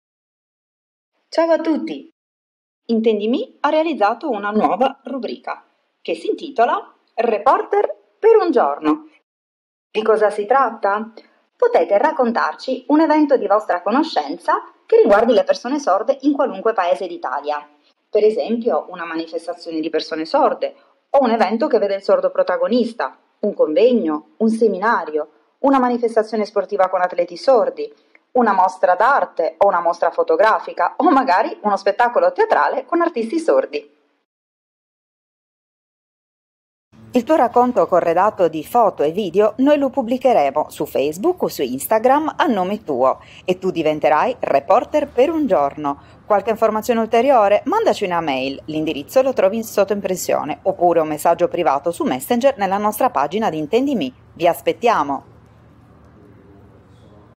Diventa "Reporter per un giorno" con IntendiMe
IntendiMe sta per lanciare la rubrica “Reporter per un giorno” per raccontare e far conoscere a tutti, gli eventi sulla sordità di ogni parte d’Italia.
Sei sordo e devi partecipare ad un evento legato alla sordità? Diventa “Reporter per un giorno” e raccontacelo con foto e video, noi lo ripubblicheremo con la tua firma!